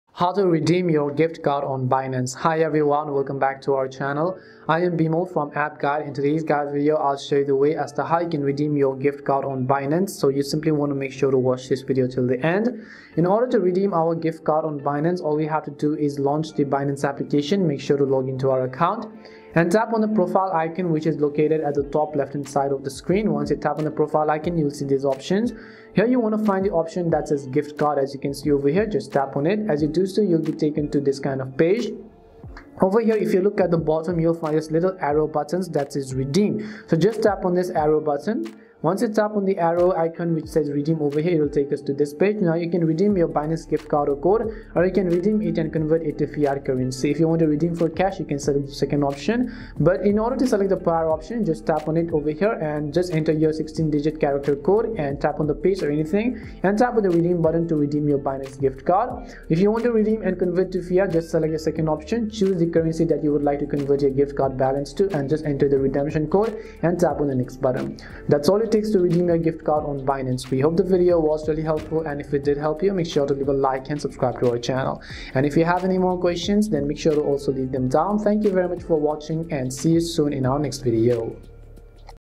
How to Redeem Gift Card on Binance !
➜ This video guides you through an easy step-by-step process to redeem a gift card on Binance. So make sure to watch this video till the end.

Follow these simple steps:
1) Update the Binance app to the latest version
2) Open Binance and login to your account
3) Tap on the profile icon from the top left
4) Choose the Gift card option
5) Go to the Redeem page
6) Choose how you would like to redeem
7) Apply the changes
8) That's all it takes to redeem your gift card on Binance.

~ Timestamps:
0:00 Introduction
0:13 Update / Login
0:19 How to Redeem Gift Card on Binance
1:08 Conclusion